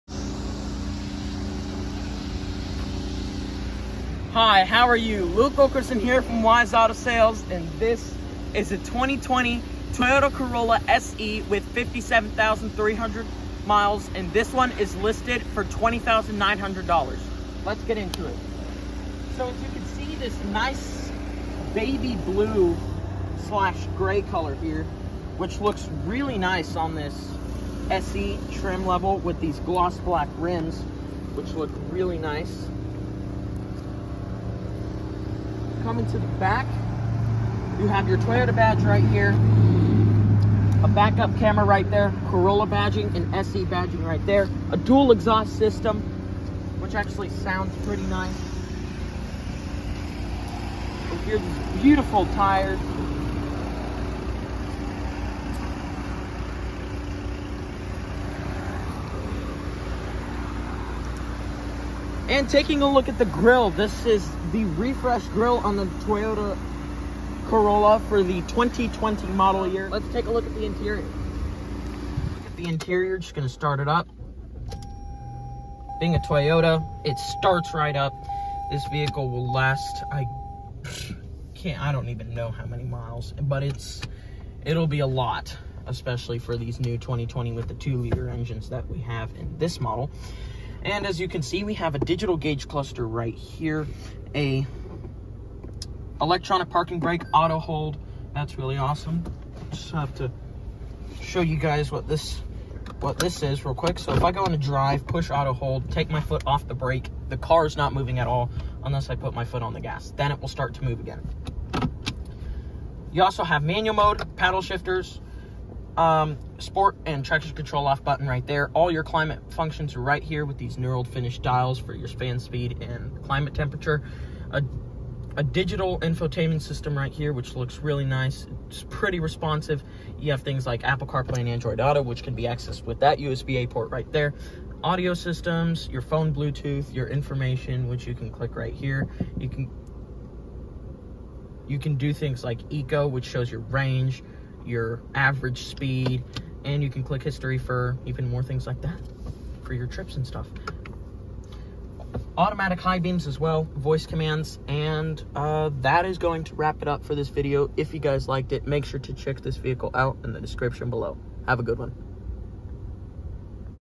2020 Toyota Corolla SE for $20,900 at Wise Auto Sales in Magnolia, Texas
This 2020 Toyota Corolla SE is listed for $20,900 and has 57,300 miles. Rebuilt title